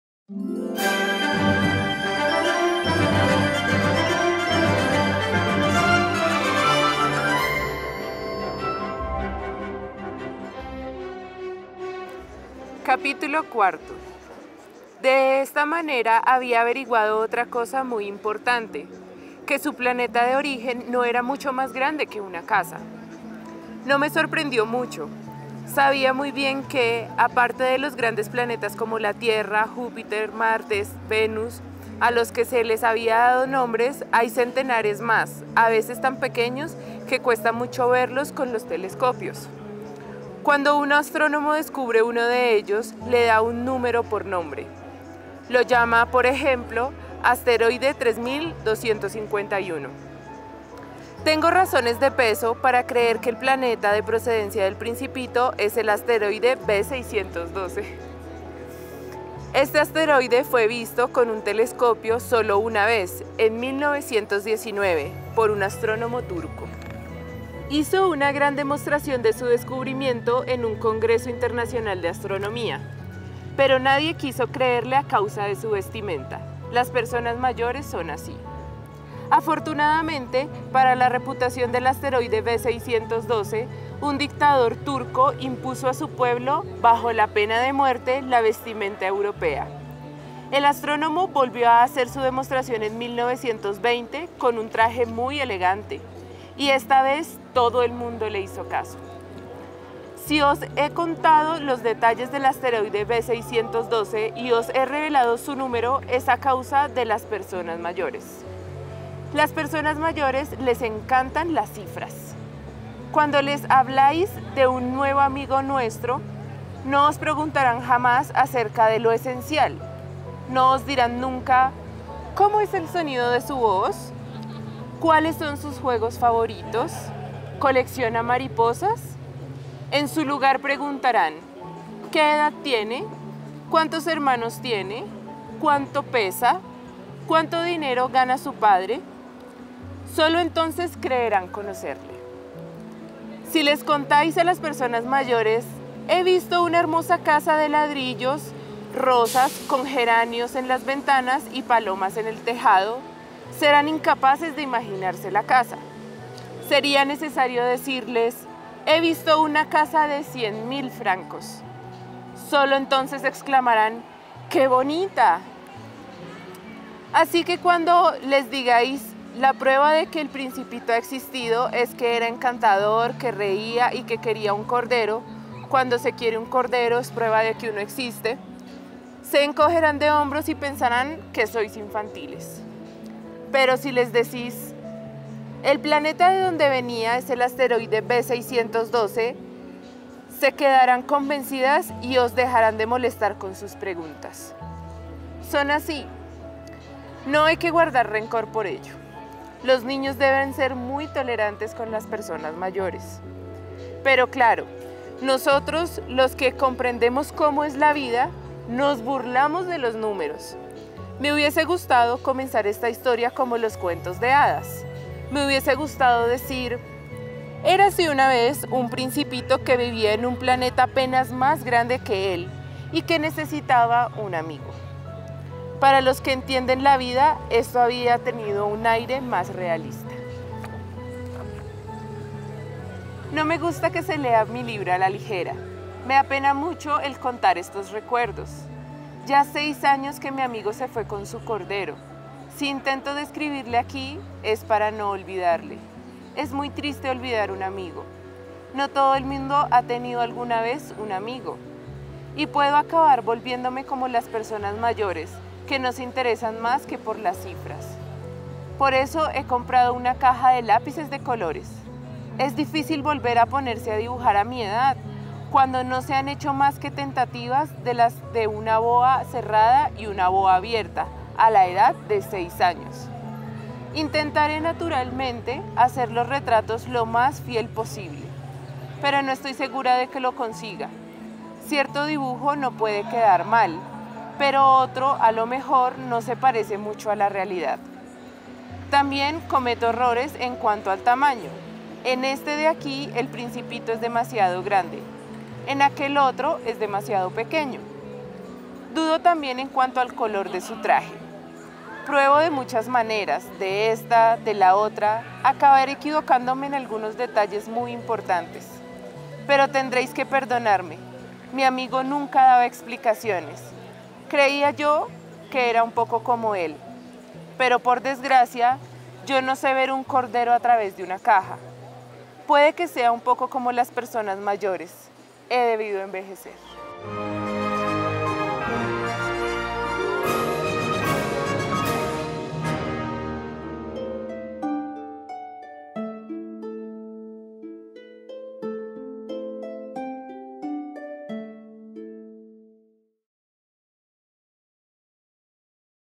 "El Principito" Antoine de Saint-Exupéry - Capítulo IV
'El Principito', del autor francés Antoine de Saint-Exupéry, cumple 80 años en 2023. Este libro se publicó por primera vez en Nueva York, Estados Unidos, el 6 de abril de 1943. Capítulo IV - Estefanía Gutiérrez Estudiante Programa en Pedagogía UPN, Integrante Proyecto Pedagógico Raíces y Alas